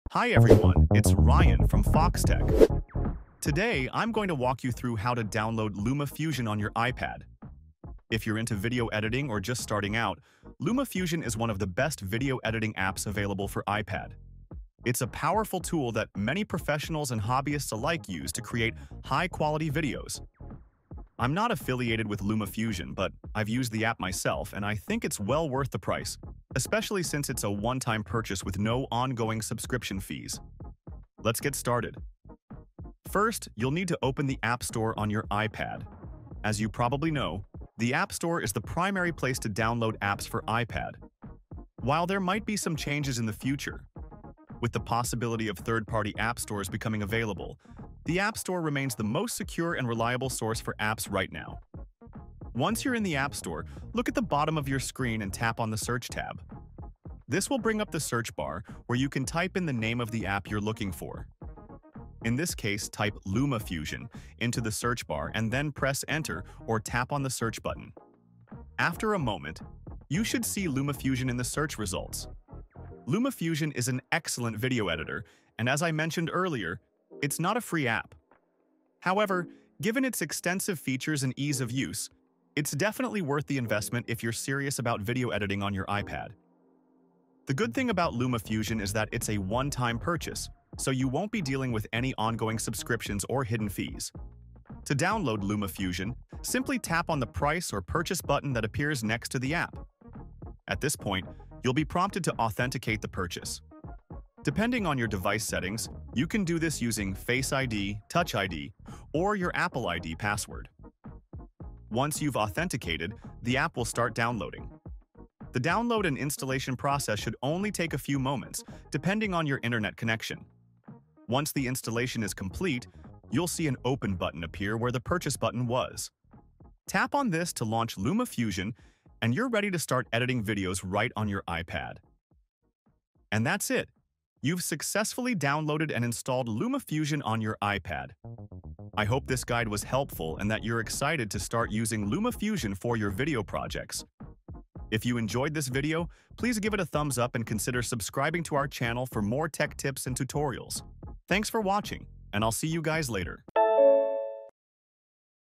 How to Download Luma Fusion on the iPad (tutorial)
In this video, we’ll guide you through the process of downloading and installing LumaFusion on your iPad. Follow along as we show you each step to get this powerful video editing app ready for your projects. Perfect for anyone looking to enhance their video editing capabilities on an iPad.